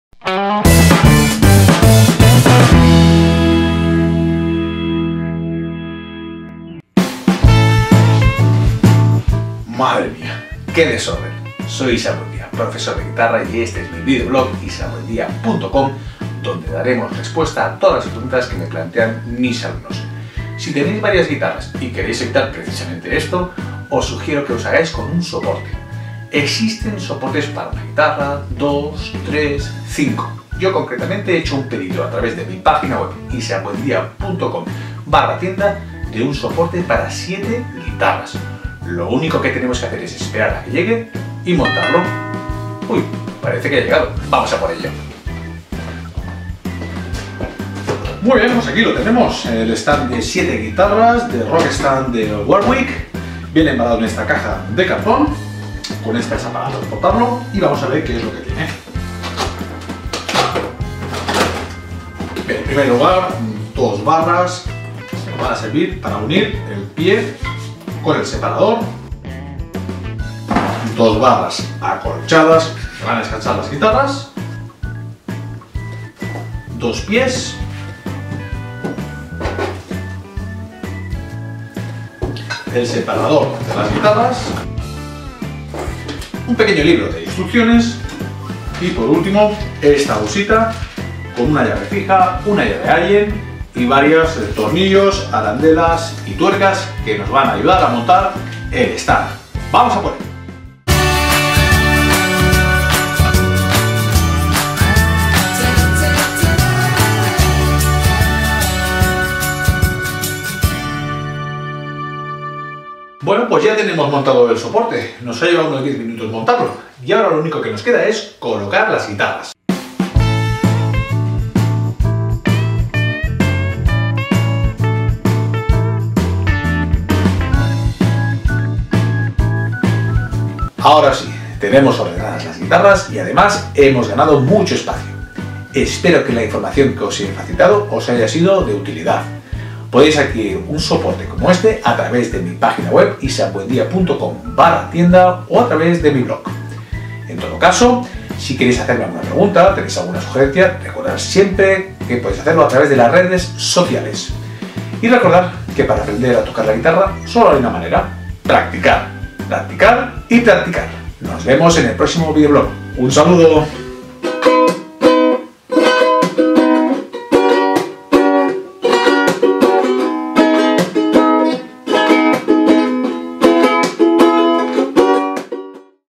👉 🎸SOPORTE DE GUITARRA 7 instrumentos: ROCKSTAND de WARWICK🎸
🎁 ¡ PUEDES ADQUIRIR TU soporte AL MEJOR PRECIO!

🙏 Gracias por colaborar y que podamos hacer más contenido en el canal a través de Paypal

¡Madre mía que desorden! Hola amigos y amigas y bienvenidos a mi videoblog dedicado al mundo de la guitarra y donde doy respuesta a las preguntas que me plantean mis alumnos. Para aquellos que dispongáis de varias guitarras o bajos y los usáis con frecuencia, os sugiero que os hagáis con un soporte para tenerlos bien ordenados. Existen soportes de una guitarra y de varias guitarras, de hecho el que he adquirido yo es para 7. Lo pedí a través de mi web Lo único que tendremos que hacer cuando llegue es montarlo.
El soporte viene embalado en esta caja con un asa. Si la abrimos lo primero que podemos ver son dos barras de metal que unirán el pie del soporte y la barra de separación de las guitarras que estará arriba. Tenemos también dos barras acolchadas donde descansaran nuestras guitarras.
Sacamos este protector de cartón  y aquí tenemos los pies y el separador de  guitarras, un pequeño libro de instrucciones, que al menos este no está en español pero si en inglés y hay unas ilustraciones que nos van ayudar, de todos modos es bastante intuitivo. Y una bolsa con tornillos, arandelas tuercas y una llave fija y una llave Allen.
Lo primero que vamos hacer es montar el pie poniendo los tornillos en estos agujeros y apretándolos con la llave allen.
En segundo lugar vamos a poner las dos barras de metal y vamos  hacer coincidir los agujeros de las dos pestañas. Ponemos el tornillo, la arandela y la tuerca y apretamos con la llave allen ayudándonos con la llave fija.
Por último ponemos el separador y colocamos la guitarra.
De este modo las guitarras quedan mucho más ordenadas y ocupan mucho menos espacio.
Recordar que para aprender a tocar la guitarra solo hay una manera practicar, practicar y practicar.
Nos vemos en el próximo videoblog.